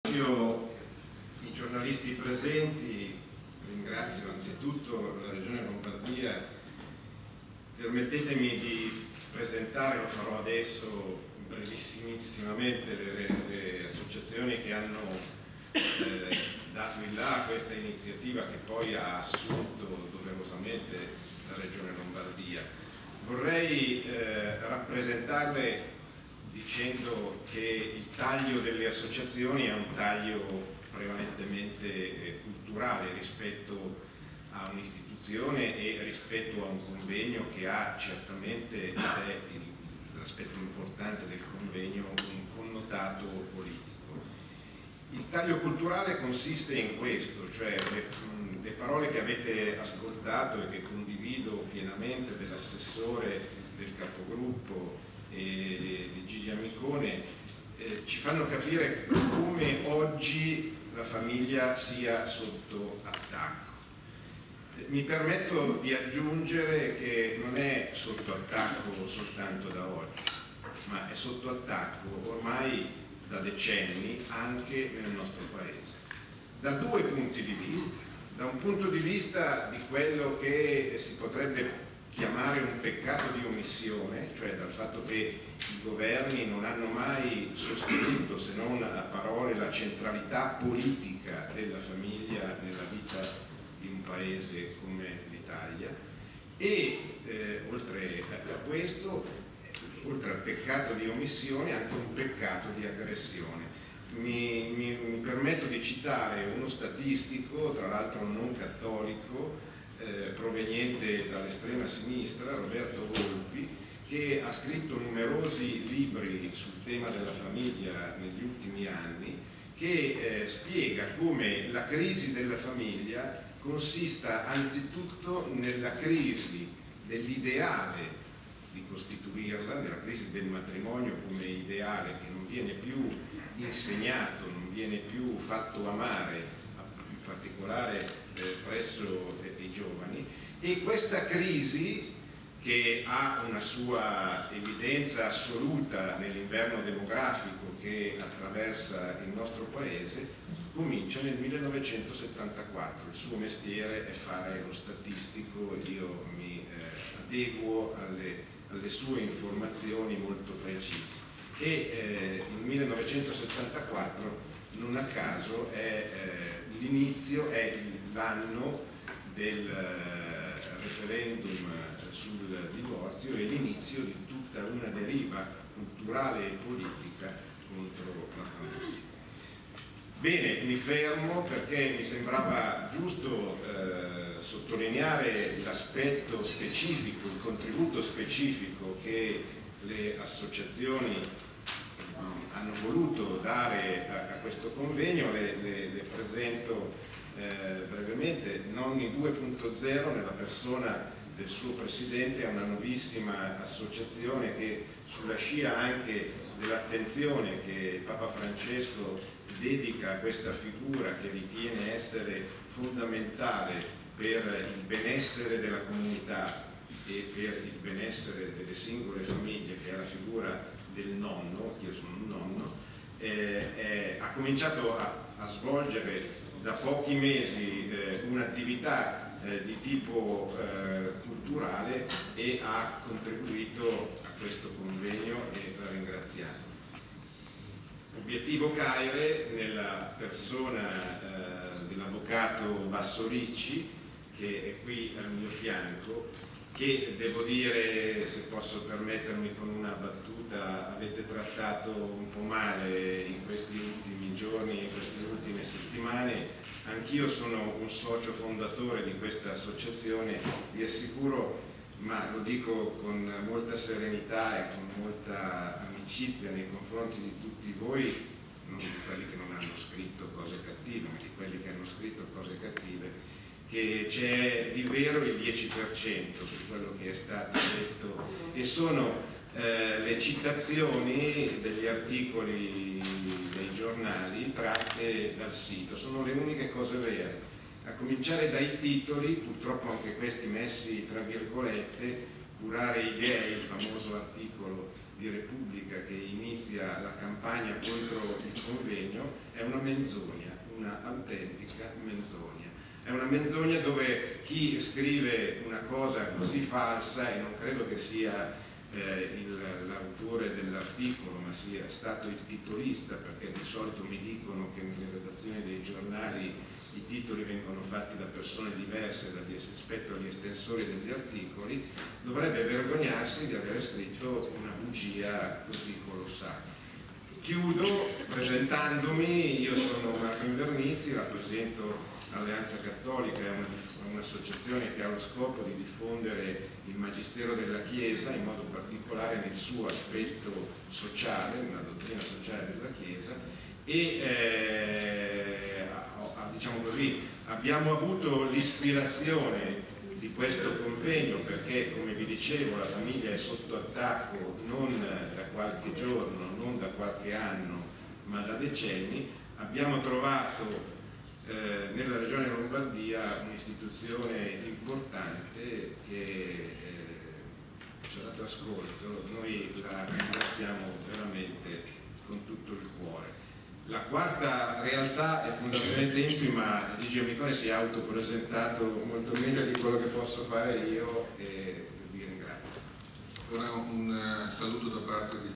Presentazione convegno famiglia - Parte quarta
Milano, 8-1-15 - Conferenza stampa di  presentazione del convegno 'Difendere la famiglia per difendere la comunità', in programma sabato 17 gennaio a Palazzo Lombardia. Parla Marco Invernizzi, Alleanza Cattolica - Parte quarta